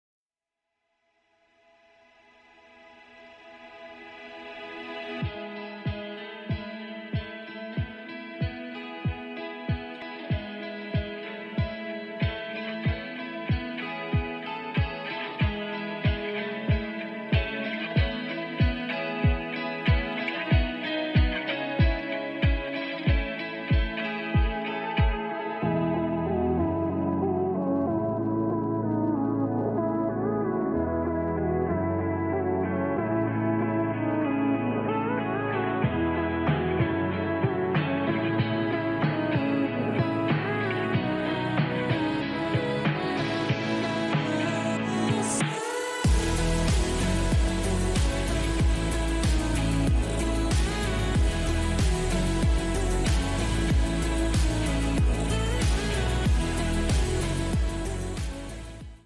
Luxiuorius Island Villa in La Bella Waters 3/1 Coral Sea Avenue, Hamilton Island, QLD 4803
La Bella Waters - a boutique development with just 8 villas, provides a private sanctuary on one of Australia's most awarded holiday island destinations.

Perfectly positioned on the much sought after northern side of Hamilton Island, in the heart of the Whitsundays this stylish two storey villa provides tranquil ocean views across Fitzalan Passage to Whitsunday island.